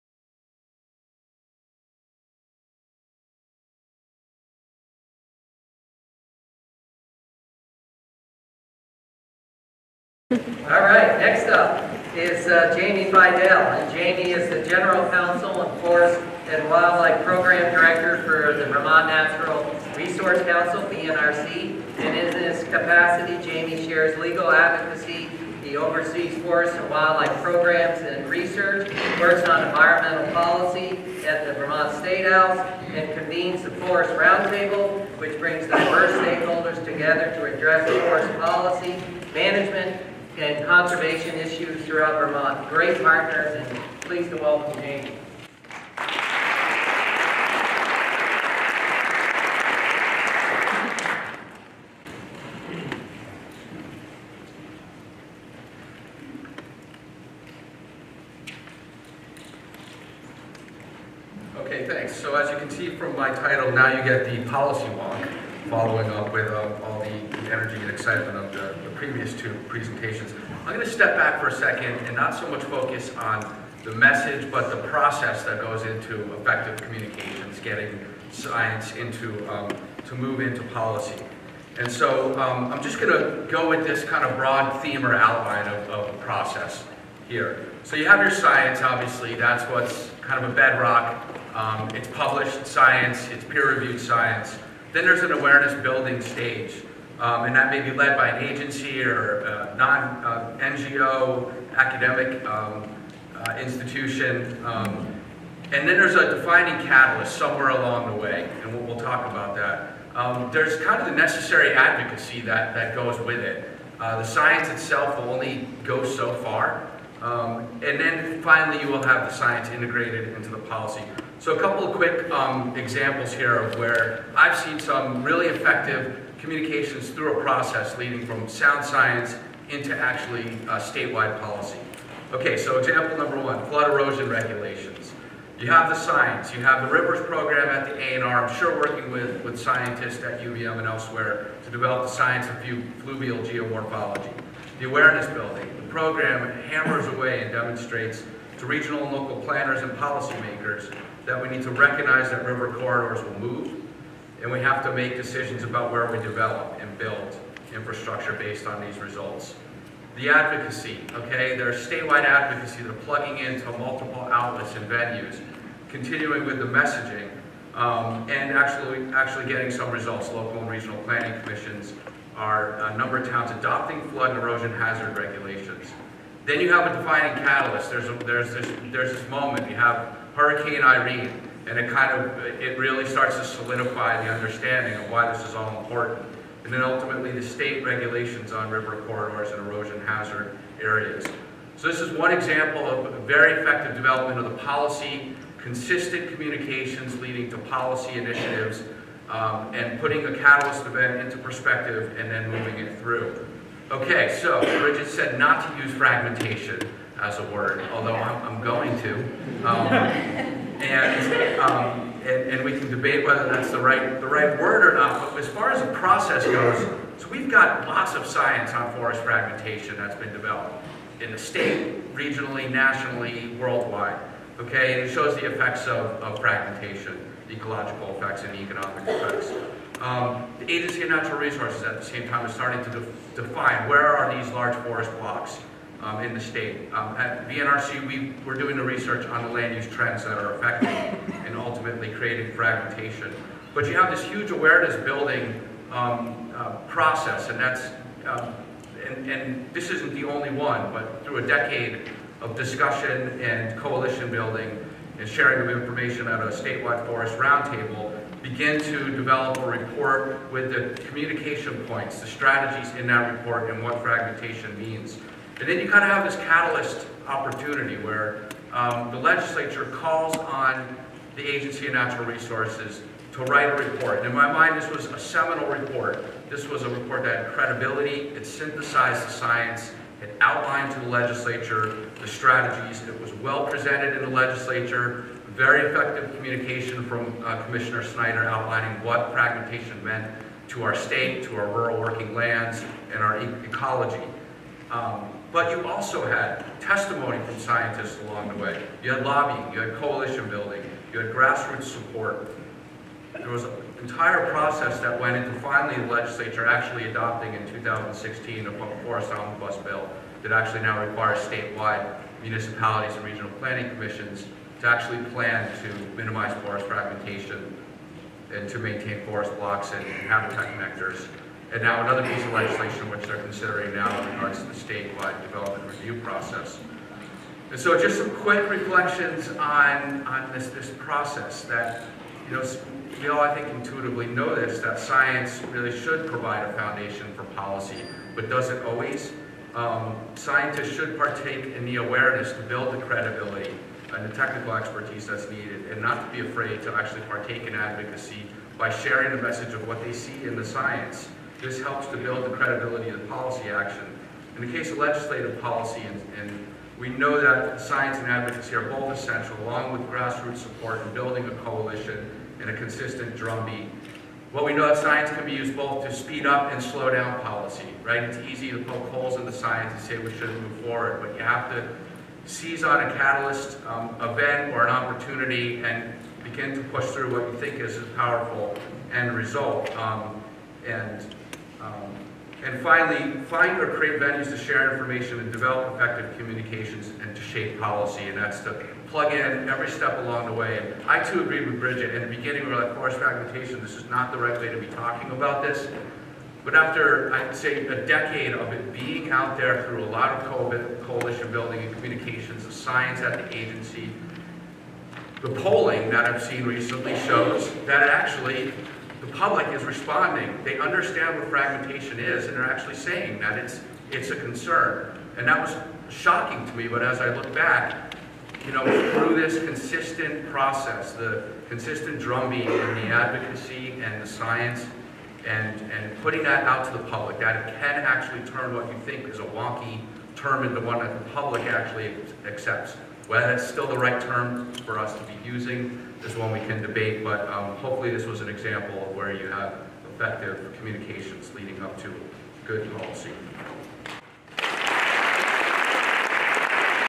Jamey Fidel Flash Talk 2017 FEMC Conference
Following our morning keynote presentation, five experts in the world of science communication gave 5-minute flash-talks synthesizing their techniques and strategies for effective communication directed towards the many stakeholders our audience will collaborate with professionally.

Reaching Decision Makers: Using Science to Inform Policy was presented by Jamey Fidel, General Counsel & Forest and Wildlife Program Director, Vermont Natural Resources Council

Presentation at the 2017 Forest Ecosystem Monitoring Cooperative Conference, more info available